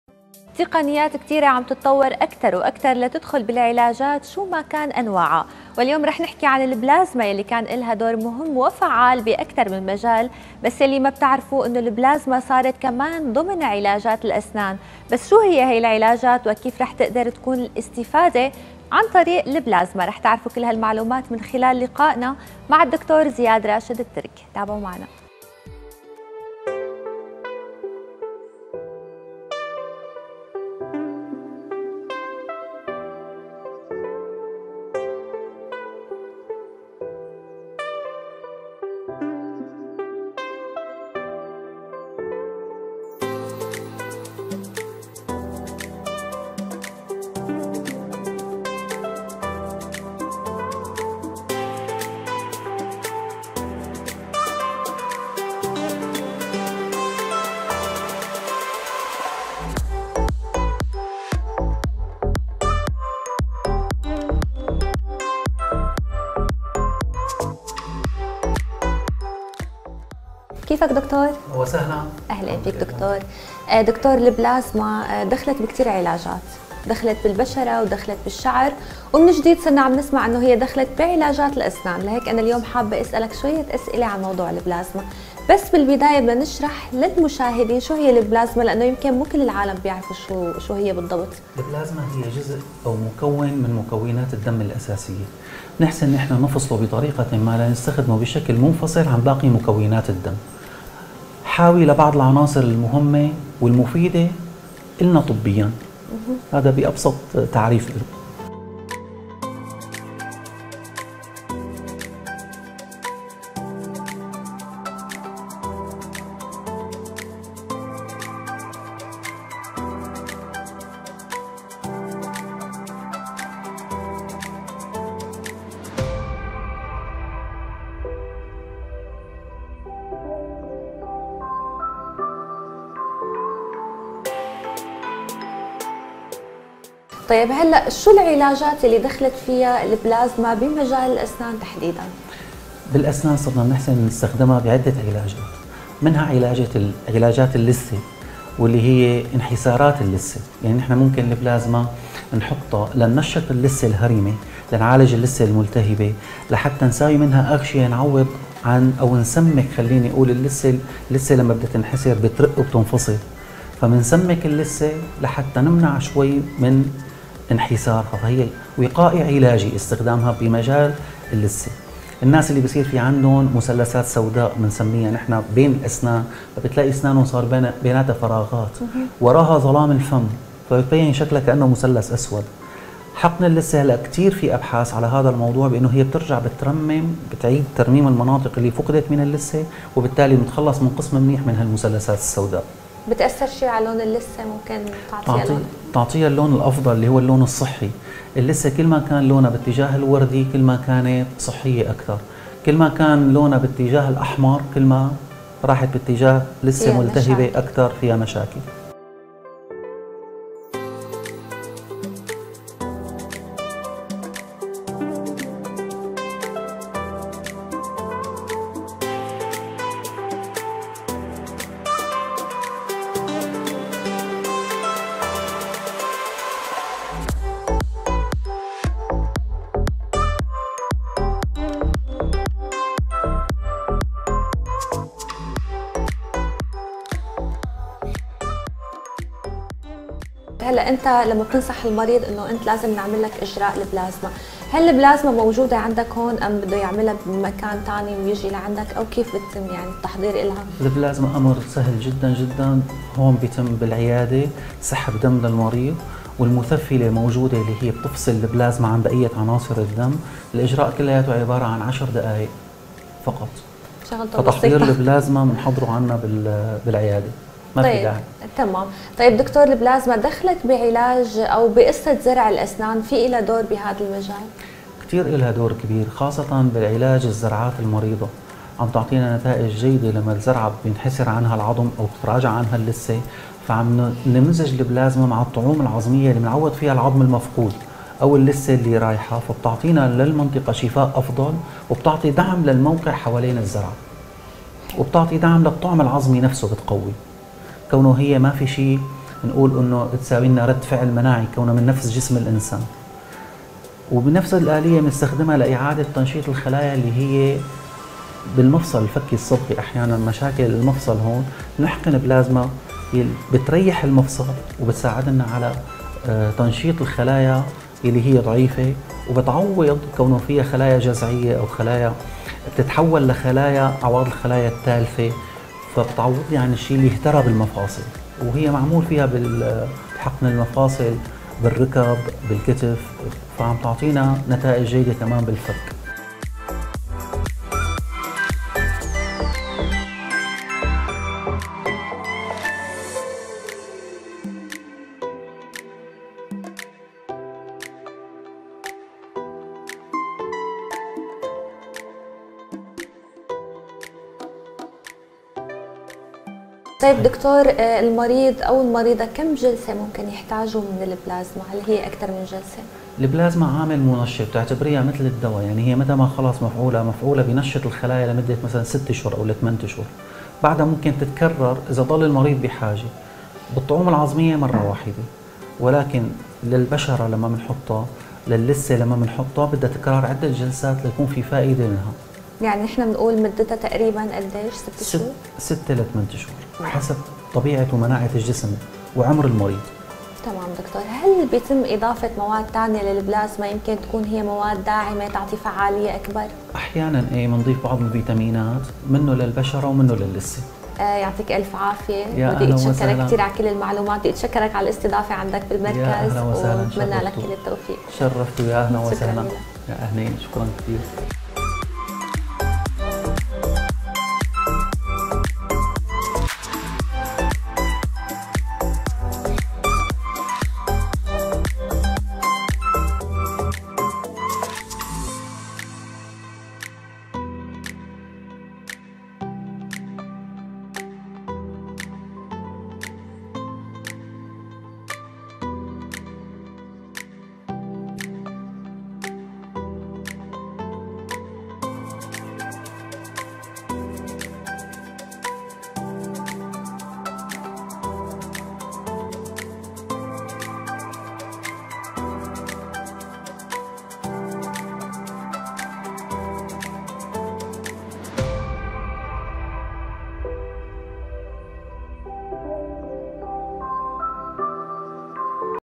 البلازما صار ضمن علاجات الأسنان خلونا نعرف أكثر عنه بهالتقرير
زياد راشد الترك - اختصاصي في جراحة الوجه و الفم و الفكين و زراعة الأسنان